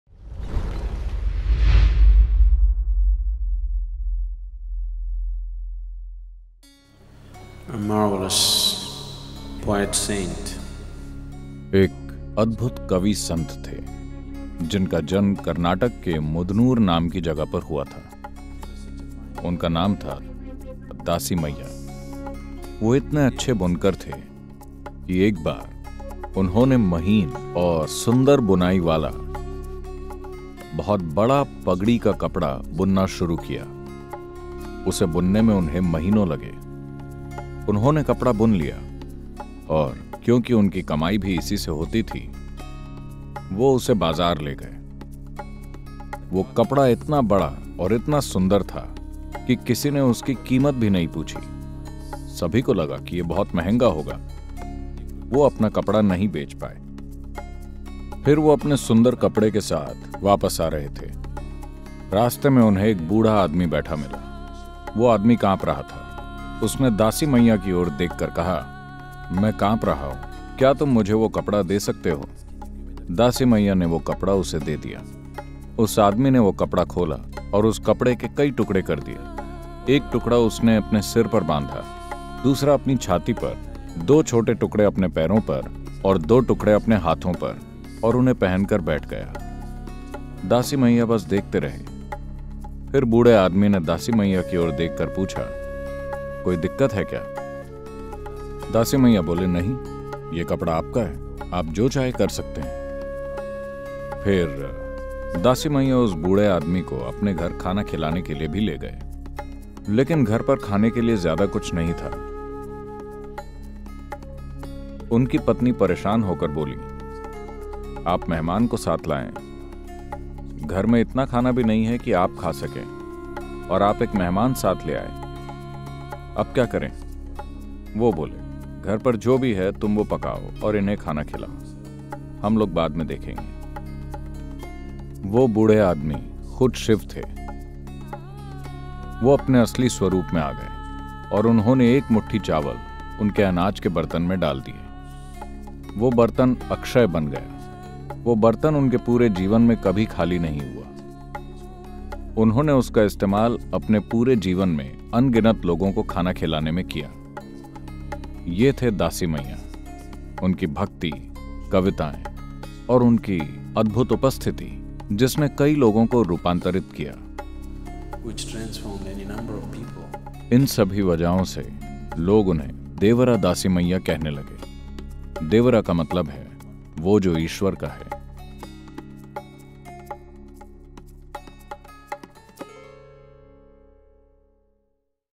देवर दासिमय्या – खुद की चिंता से मुक्‍त, एक अद्भुत संत। Devara Dasimaya [Hindi Dub]
दासिमय्या एक बहुत अच्छे बुनकर थे। एक बार उन्होंने महीन और सुंदर बुनाई वाला एक बहुत बड़ा पगड़ी का कपड़ा बुना, जिसे बुनने में उन्हें महीनों लग गए। फिर वे उसे बाजार ले गए। वो कपड़ा इतना बड़ा और इतना सुन्दर था कि किसी ने उसकी कीमत भी नहीं पूछी – सभी को लगा कि ये बहुत महँगा होगा। वो अपना कपड़ा नहीं बेच पाए।  फिर वो अपने सुन्दर कपड़े के साथ वापस आ रहे थे। रास्ते में उन्हें एक बूढ़ा आदमी बैठा मिला – वो आदमी काँप रहा था। उसने दासिमय्या की और देख कर कहा – “मैं काँप रहा हूँ। क्या तुम मुझे वो कपड़ा दे सकते हो?” दासिमैय्या ने वो कपड़ा उसे दे दिया। उस आदमी ने वो कपड़ा खोला और उस कपड़े के कई टुकड़े कर दिए। एक टुकड़ा उसने अपने सिर पर बाँधा, दूसरा अपनी छाती पर, दो छोटे टुकड़े अपने पैरों पर, और दो टुकड़े अपने हाथों पर। उसने कपड़े के कई टुकड़े कर दिए और उन्हें पहनकर बैठा गया। दासिमय्या बस देखते रहे।

फिर क्या हुआ? ...आईये, पूरी कहानी सुनते हैं सद्‌गुरु से।